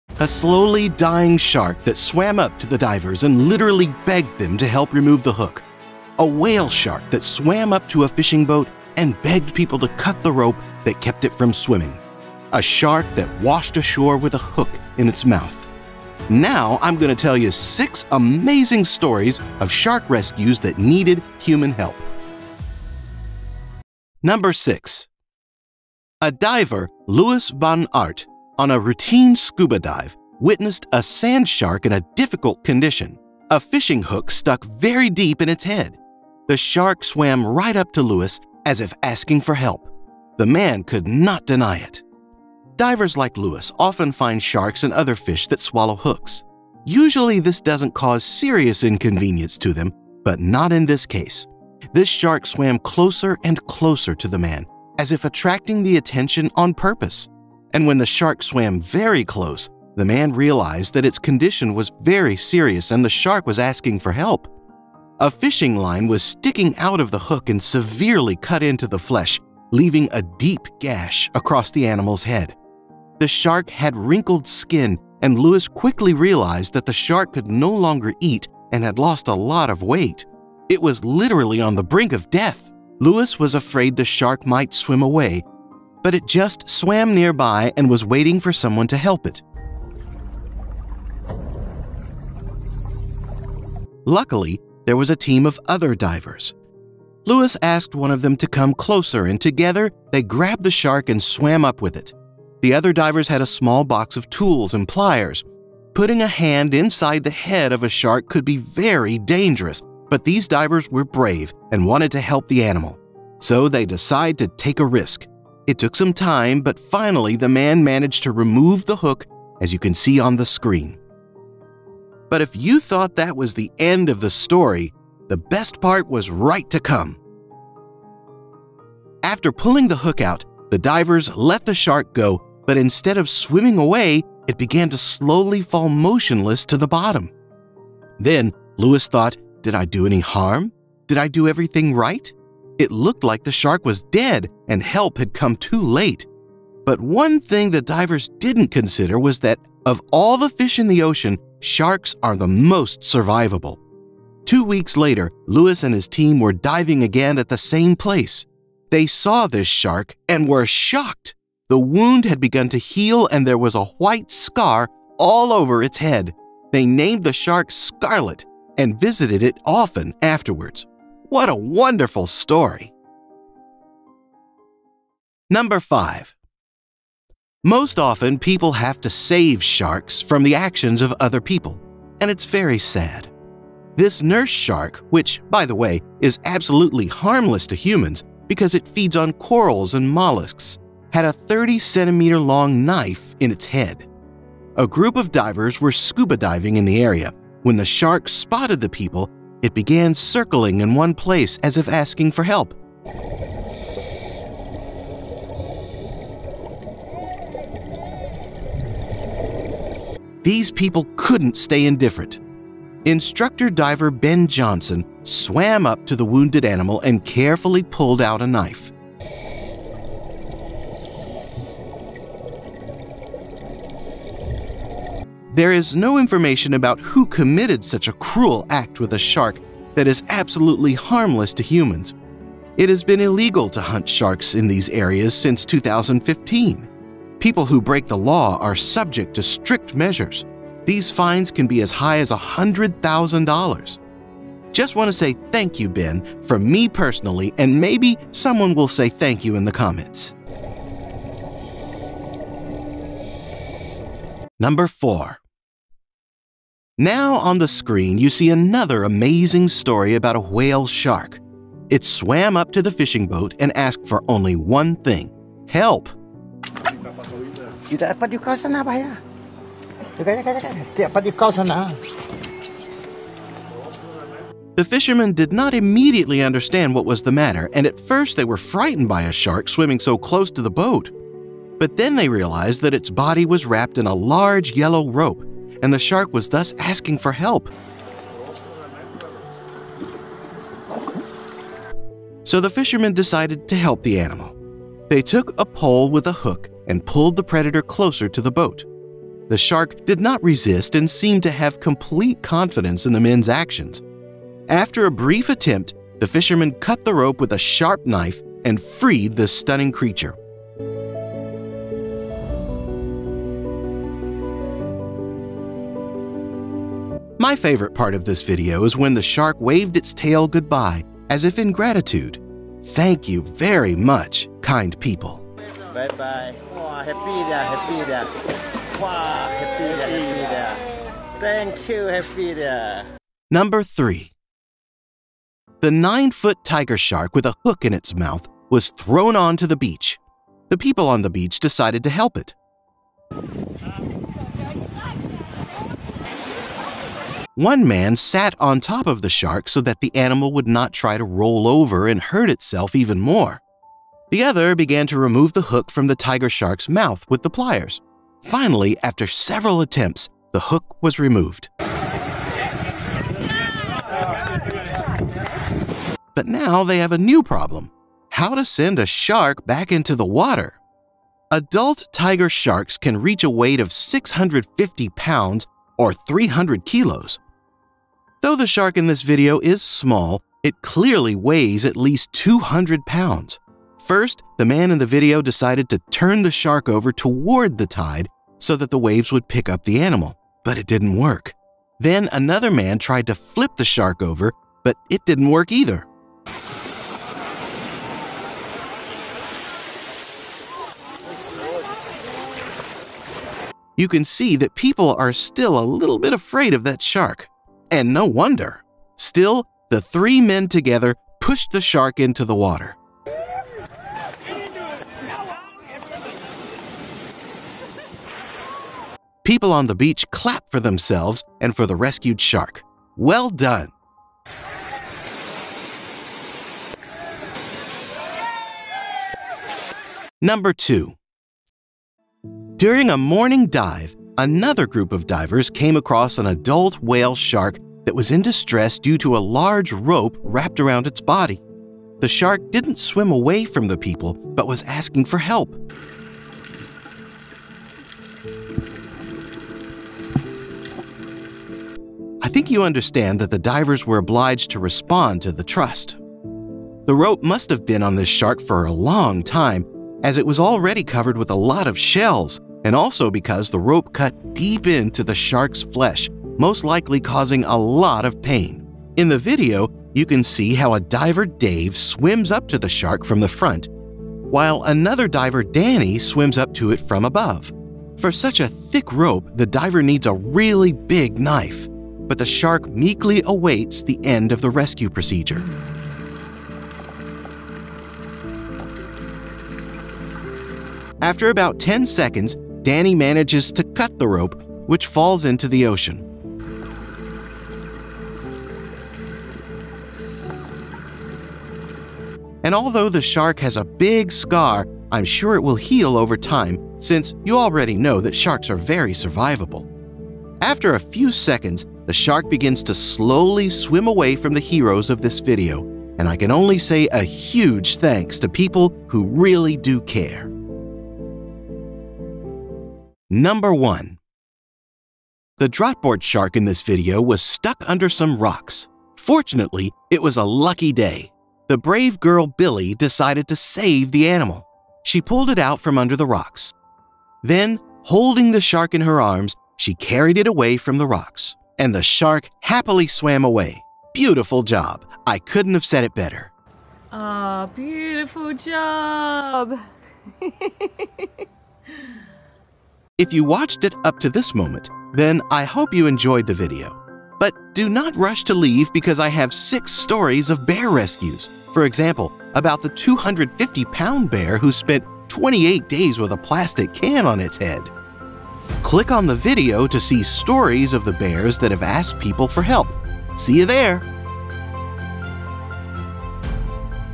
Sharks That Asked People for Help & Kindness
A slowly dying shark that swam up to the divers and literally begged them to help remove the hook. A whale shark that swam up to a fishing boat and begged people to cut the rope that kept it from swimming. A shark that washed ashore with a hook in its mouth. Now I'm gonna tell you 6 amazing stories of shark rescues that needed human help.
Timestamps:

00:00 Overview
00:24 - 6 - Shark asks to remove the hook
02:56 - 5 - Shark asks to remove the knife
04:15 - 4 - Whale shark asks to cut a big rope
05:33 - 3 - Shark washed ashore with a hook in its mouth
07:11 - 2 - Whale shark asked for help
08:45 - 1 - Shark stuck under rocks